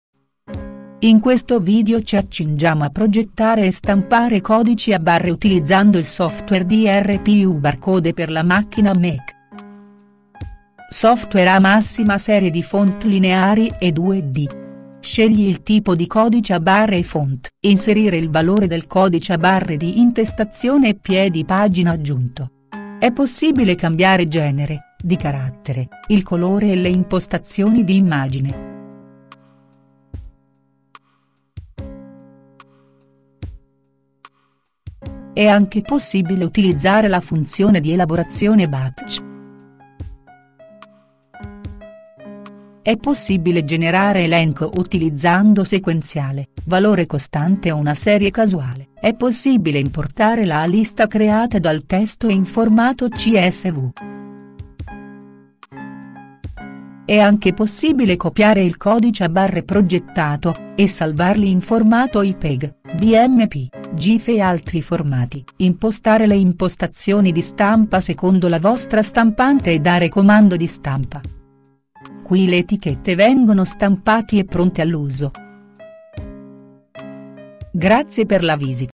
Come progettare e stampare codici a barre Etichette con Mac
Progettazione e stampa di codici a barre etichette in diverse forme e dimensioni secondo il requisito con macchine Mac. Creare etichette barcode e adesivi per le diverse industrie come l'imballaggio, fornitura, sanità, retail e molti altri.